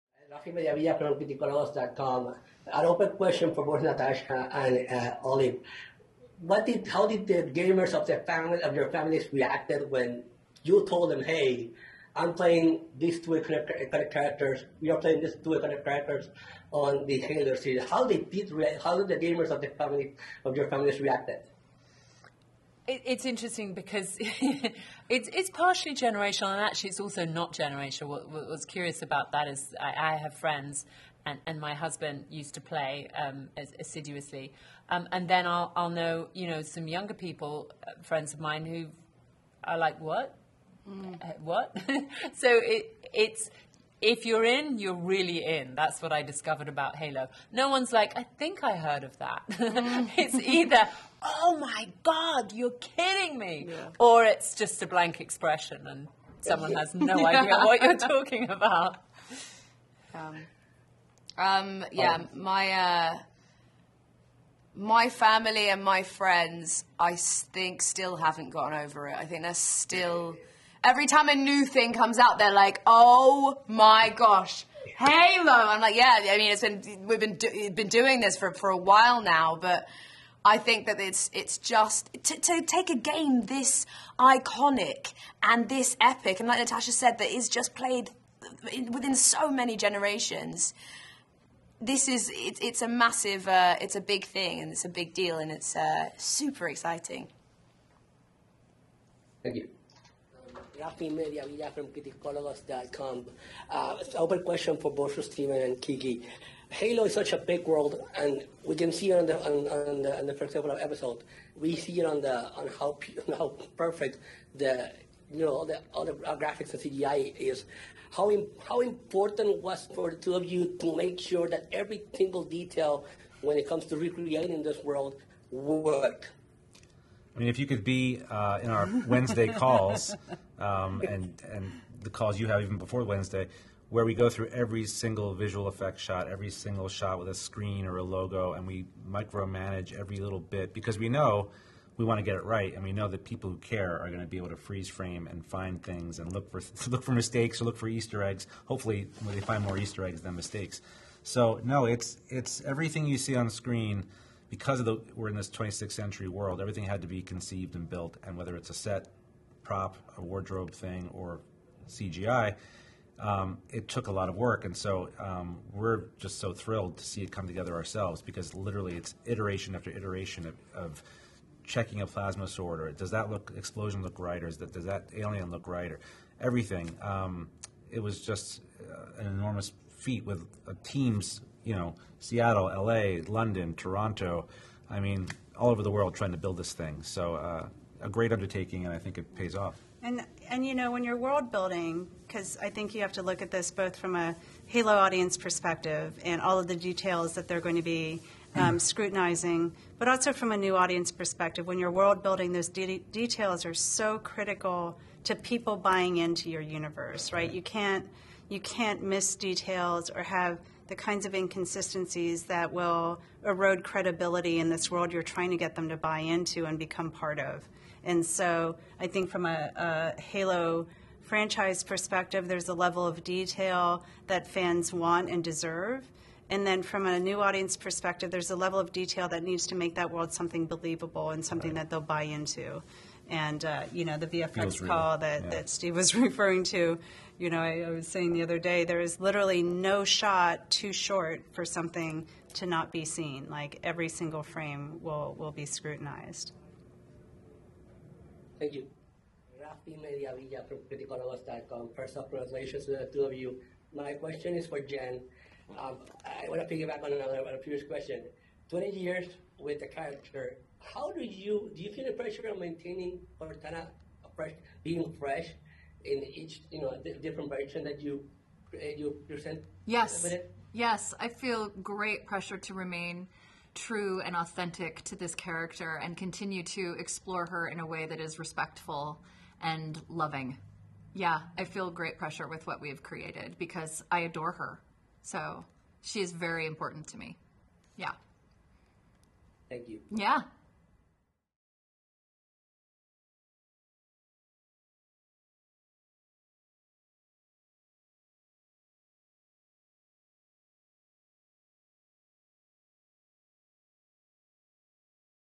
Interviews w/ The Cast & Creators Of Paramount Plus "Halo" (The Series) - Out March 24.
In its adaptation for Paramount+, HALO will take place in the universe that first came to be in 2001, dramatizing an epic 26th-century conflict between humanity and an alien threat known as the Covenant.

Starring Pablo Schreiber, Natascha McElhone, Jen Taylor, Bokeem Woodbine, Shabana Azmi, Natasha Culzac, Olive Gray, Yerin Ha, Bentley Kalu, Kate Kennedy, Charlie Murphy, Danny Sapani, Ryan McParland, Burn Gorman, Fiona O’Shaughnessy

Directed by Otto Bathurst

Executive Producers Steven Spielberg, Steven Kane, Darryl Frank, Justin Falvey, Otto Bathurst, Toby Leslie, Kyle Killen, Scott Pennington, Kiki Wolfkill, Frank O’Connor, Bonnie Ross